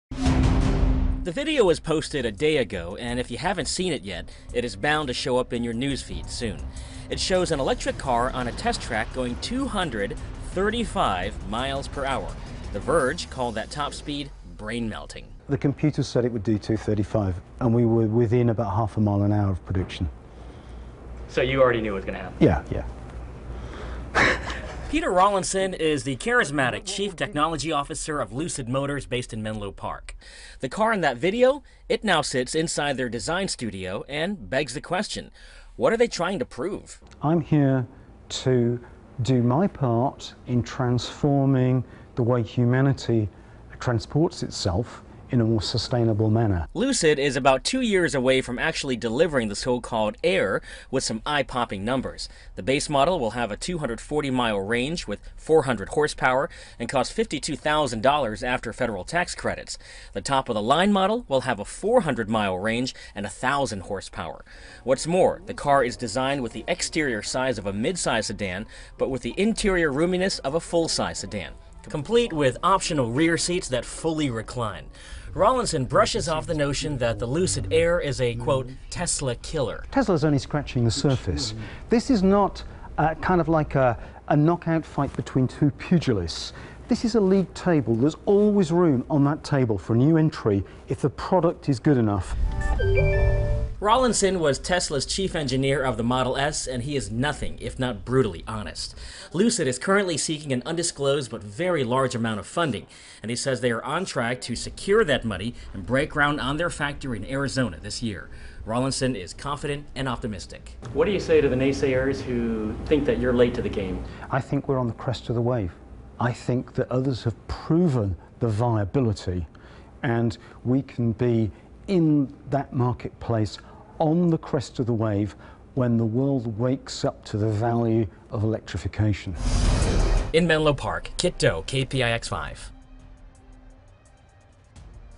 New High-End Electric Car Produced In Menlo Park To Compete With Tesla
Kiet Do reports on Lucid Motors producing an electric car that can go over 220 miles per hour (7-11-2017)